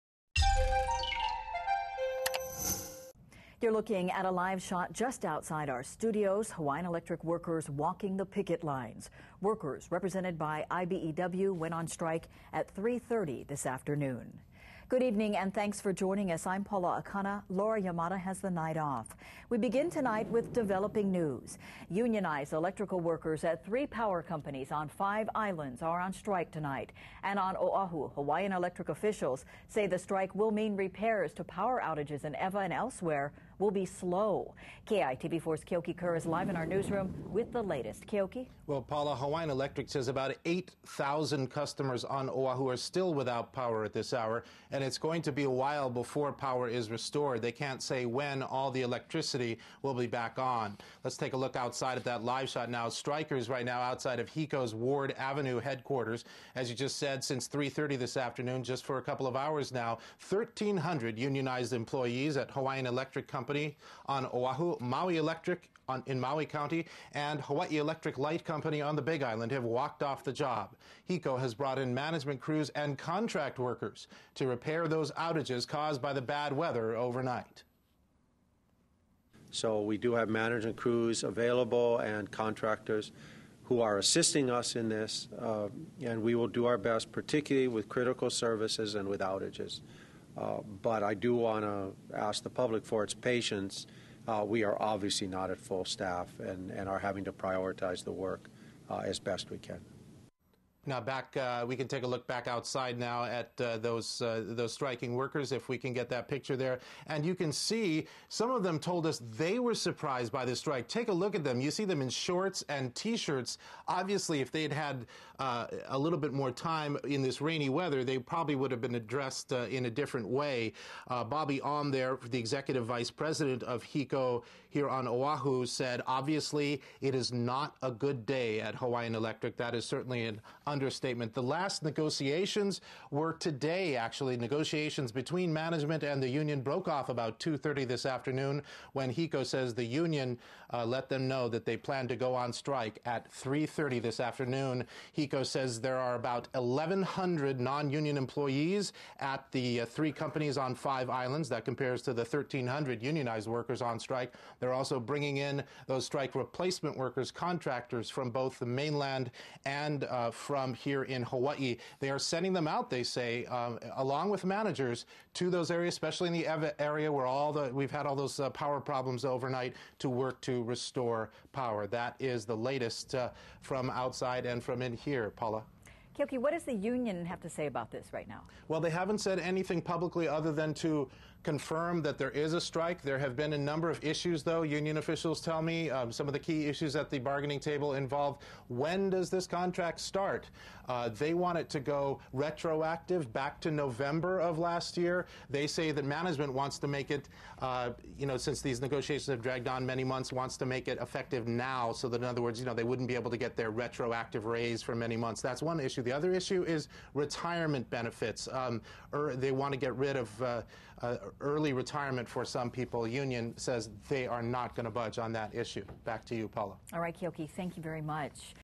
HECO Workers Go On Strike
In the midst of power problems Hawaiian Electric workers went on strike. For news you need to know,